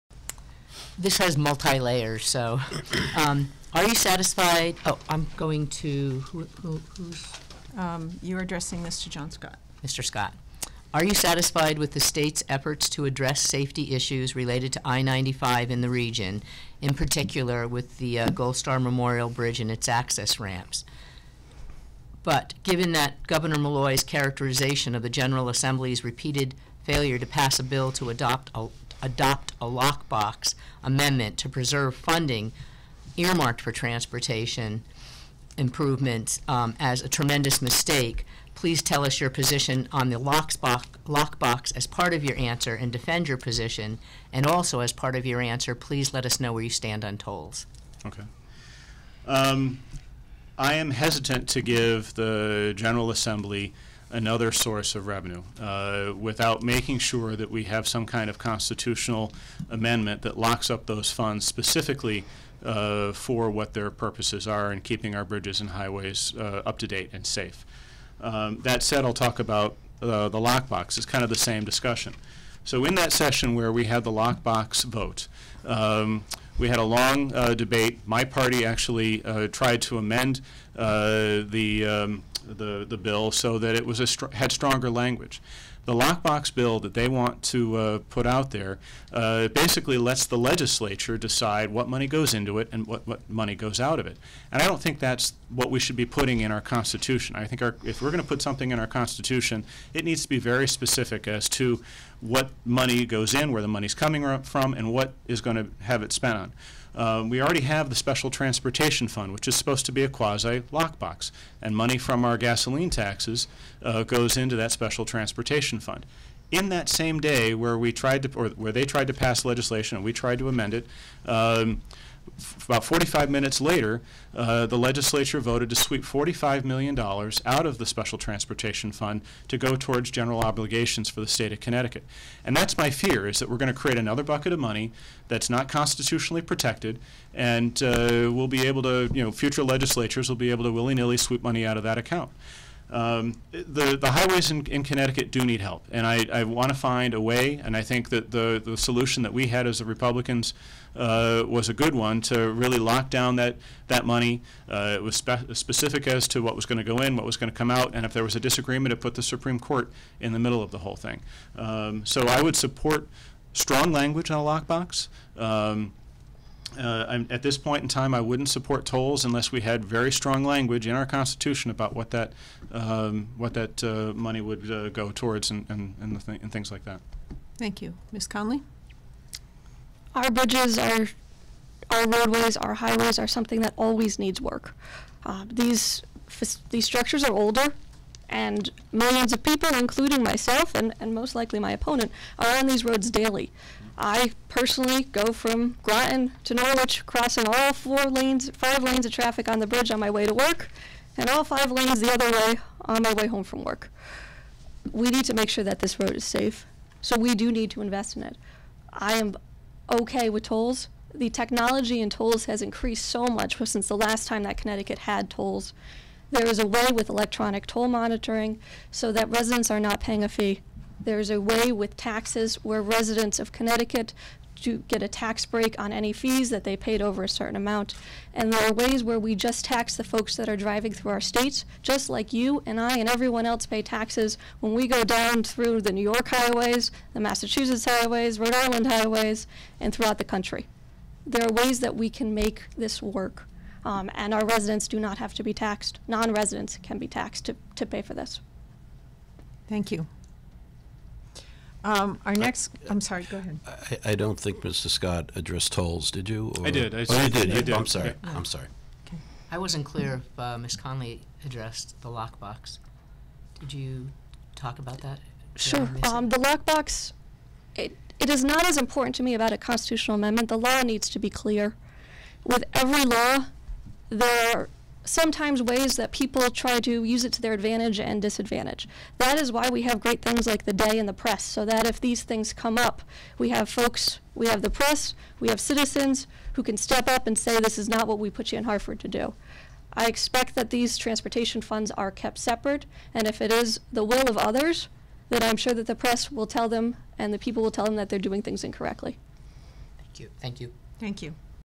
5. What is your opinion on the transportation lockbox and tolls on I-95?
Democrat Christine Conley and incumbent Republican John Scott are running for the 40th House District seat representing Groton and Ledyard.